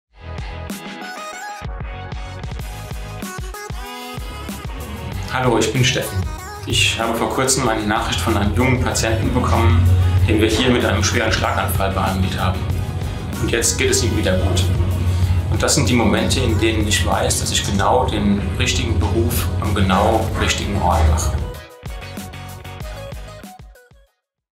Raum für alle, Platz für dich - Steffen aus dem Team Winterberg
Steffens Weg zum Arztberuf verlief alles andere als geradlinig.
Nach dem Abi hat sich der heutige Oberarzt nämlich für eine Ausbildung zum Bankkaufmann entschieden. Danach wurde ihm aber klar: Ich will Arzt werden. Und diesen Wunsch hat er sich erfüllt. Seit 12 Jahren ist er Teil unseres Team Winterberg und liebt sein Fachgebiet – die Radiologie.

„Mein Beruf ist vielseitig und spannend, erfordert Konzentration und Fachkompetenz. Das macht mir extrem viel Spaß, ich kann mir keinen besseren Beruf und keinen besseren Ort dafür vorstellen“.

Du willst Teil des Team Winterberg werden? Wir haben Platz für dich

Übrigens: Alle Motive, Texte und Bilder unserer neuen Kampagne bewegdich findet ihr nach und nach